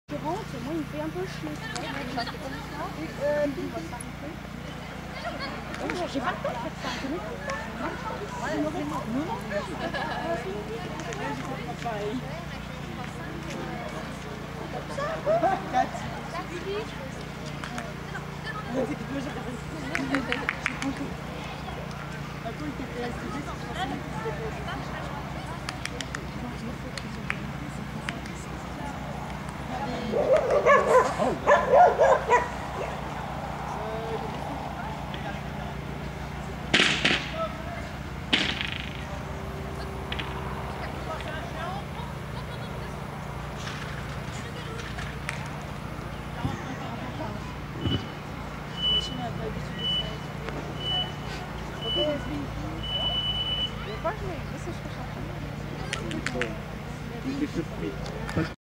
Aschka manche agility 2e degré cat B ChR Bourgogne 2013
Championnat de Bourgogne d'agility du 21 avril 2013 à Daix, manche 2 agility, 2e degré, catégorie B